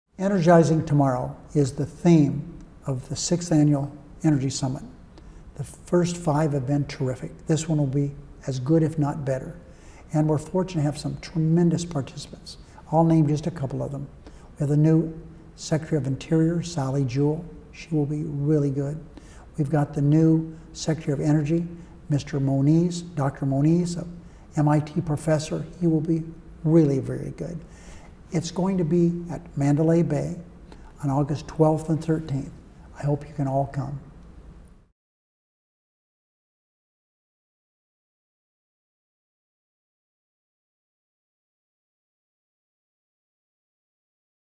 Senator Reid Announces the National Clean Energy Summit 6.0: Energizing Tomorrow
Reid announces his sixth annual Clean Energy Summit, which takes place in Nevada.